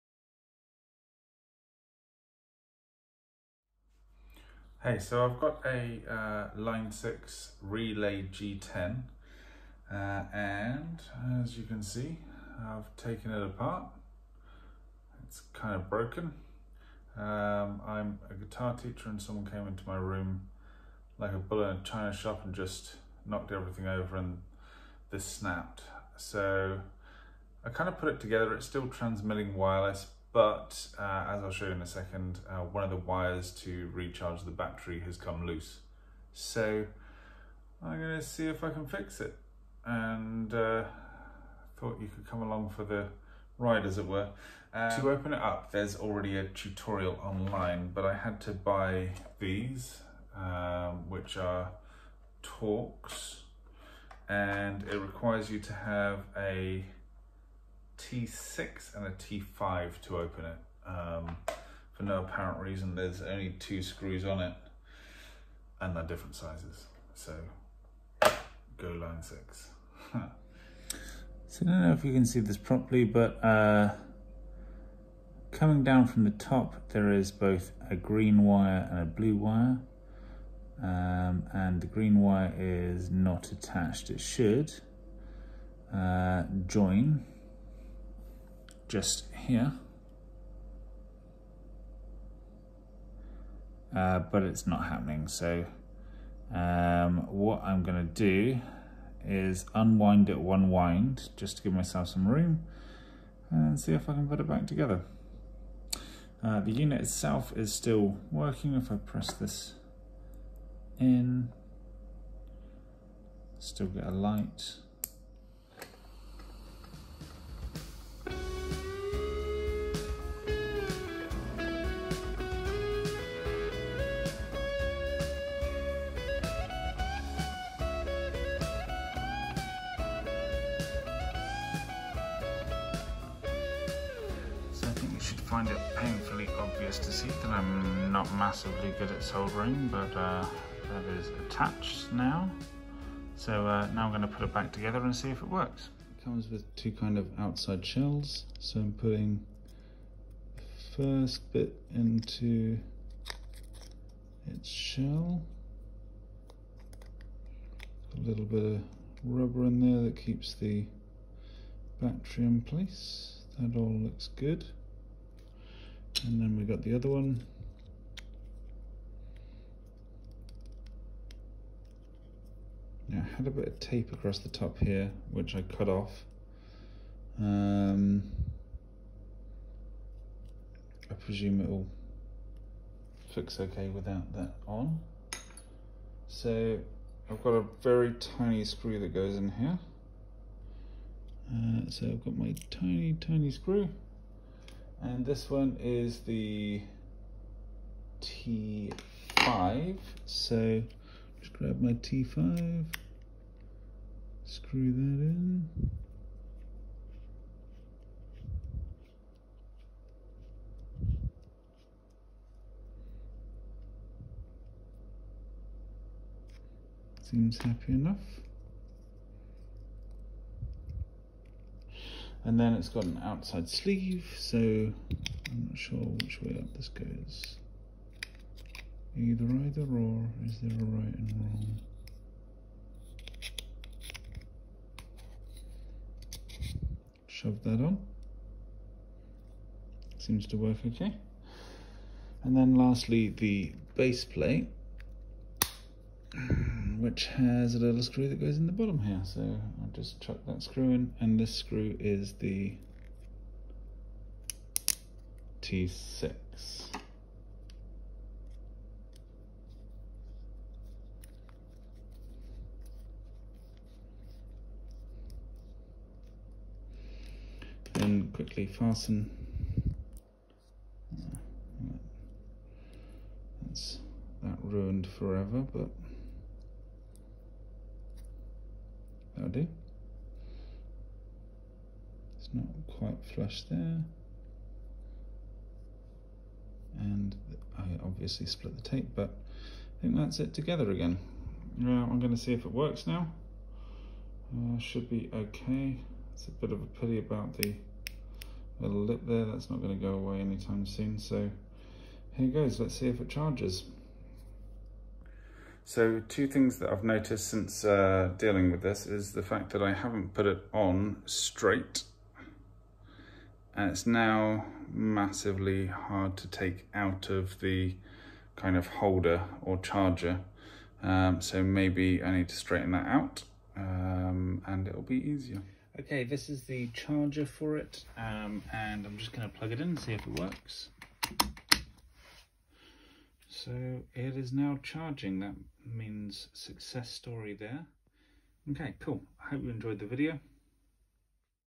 Fixing the G10 Line6
I have absolutely no technical expertise but my G10 broke and I thought I would try to fix it myself. It's pretty expensive to replace and it was a pretty straightforward job. On a side note, the G10 is an awesome piece a kit which replaces your guitar cable with wireless technology, instead of radio technology! No more lugging around a million messy cables. I absolutely love it.